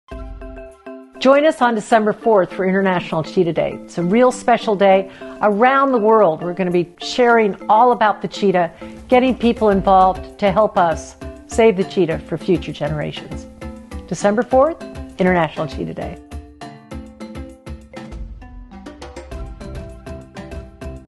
Dr. Laurie Marker on International Cheetah Day
Help spread the word to SaveTheCheetah for InternationalCheetahDay on December 4th! Tag @CCFCheetah on social media to help raise awareness for CCF's work across the species' range.